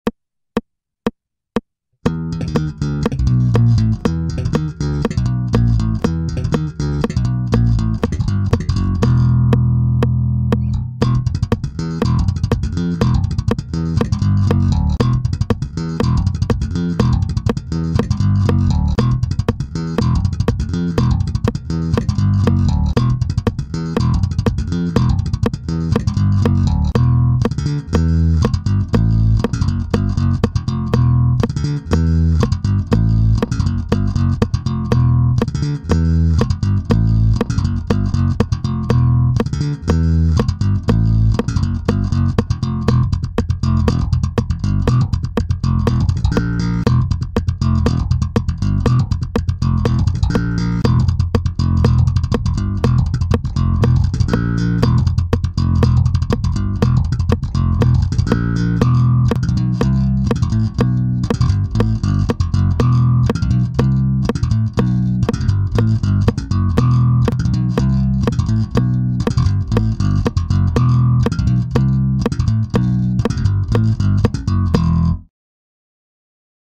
MARLEAUX CONSAT CUSTOM 5 Aleks - Inffantta
Aleksandar Zdravkov of INFFANTTA showcasing the wonderful sound and playability of a  MARLEAUX BASS